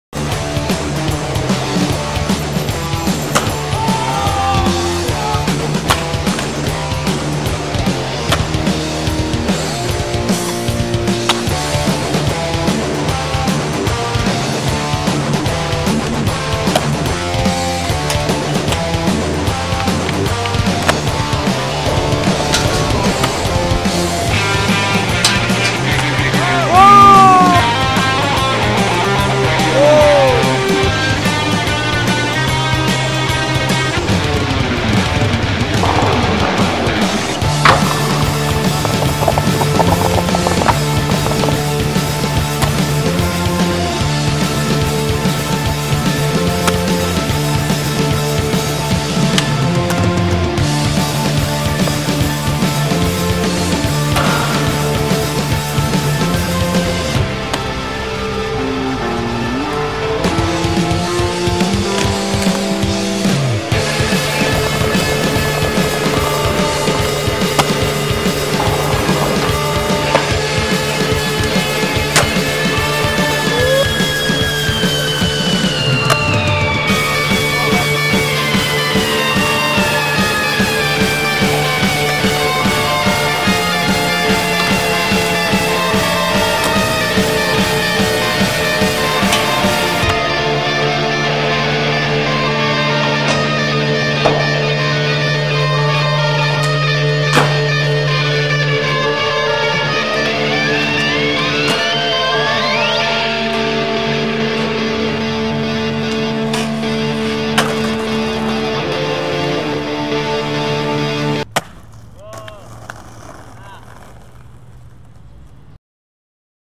juan peña 14 years old
esta part no tiene nada de especial, fue grabado hace 2 o 3 años mas o menos, y bueno consegui este demo, espero lo disfruten (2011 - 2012)